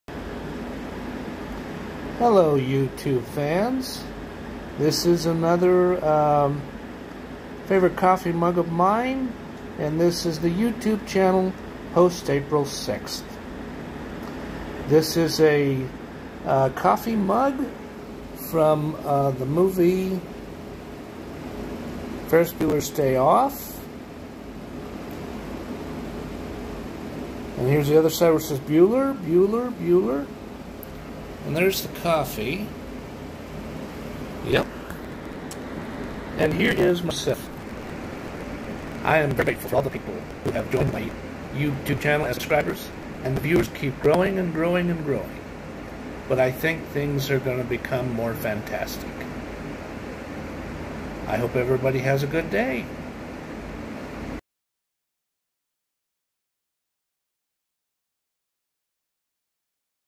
ME DRINKING A CUP OF COFFEE IN A FERRIS BUELLERS OFF MUG actor POSTAPRILSIXTH MOVIE MEMORIES
I discuss two previous guitars I owned. Between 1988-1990, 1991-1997, one of them being a fender squire electric guitar. I tried to strum many types of guitar songs. I turn the guitar upside down and tap on it like a drum. And I smoke a Custom-Built pipe to with a long shank.